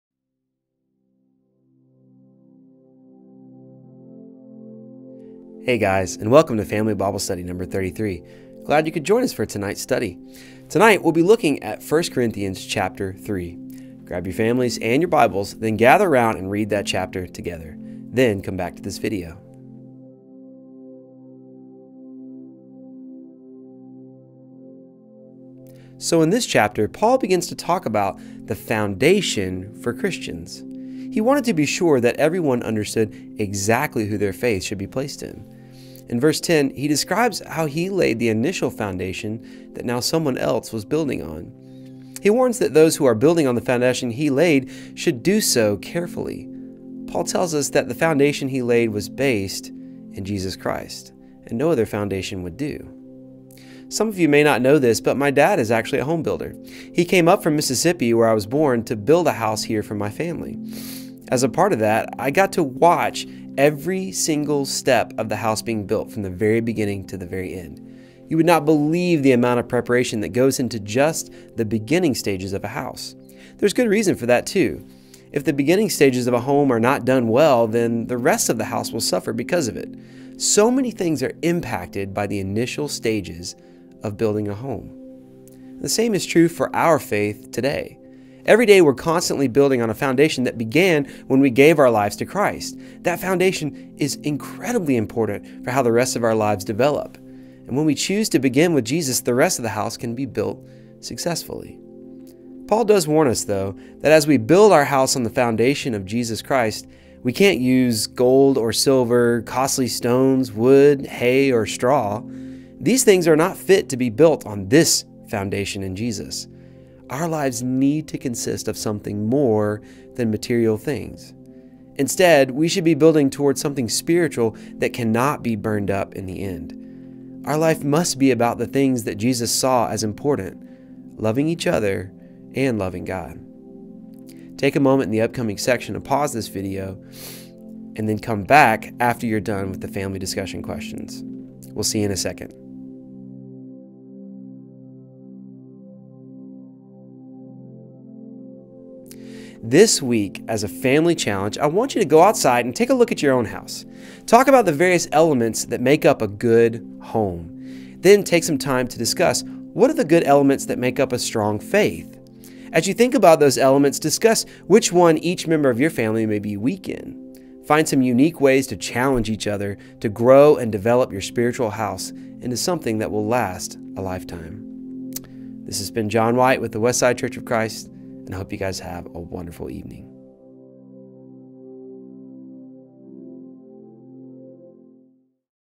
Family Bible Study #33 // 1 Corinthians 3 // 11-2-20
Join us for a family oriented Bible study with discussion questions! It only takes a few minutes to train your kids up in the way they should go!